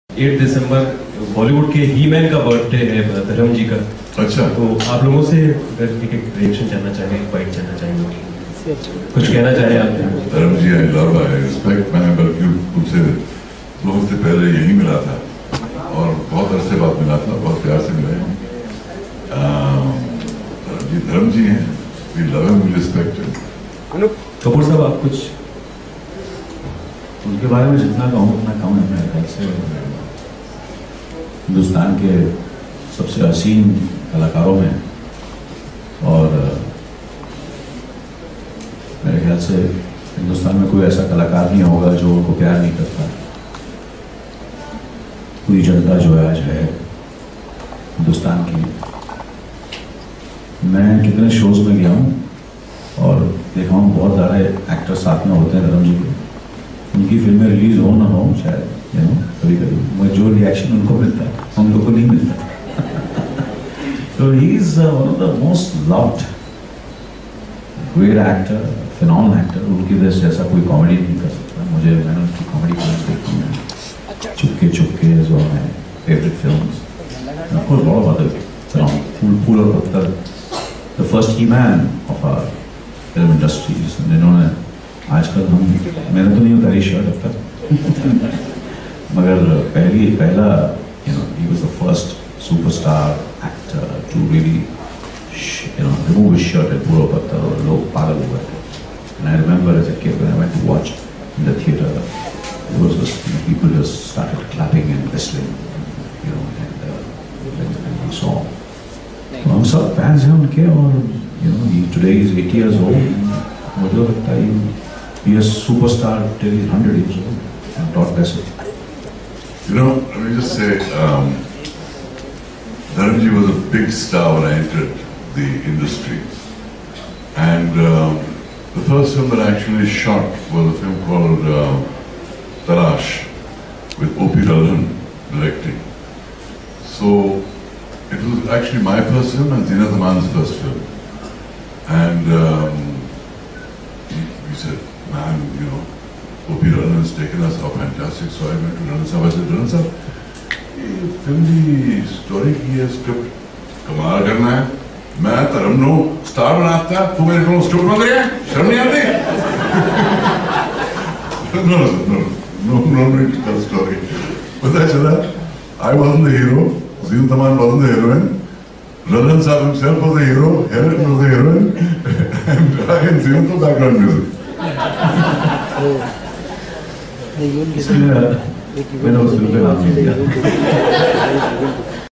Anil Kapoor, Kabir Bedi wish Dharmendra on his birthday
Veteran actor Dharmendra turned 80 on Tuesday and his Bollywood colleagues Anil Kapoor and Kabir Bedi used the platform at an event in Mumbai to wish the legend.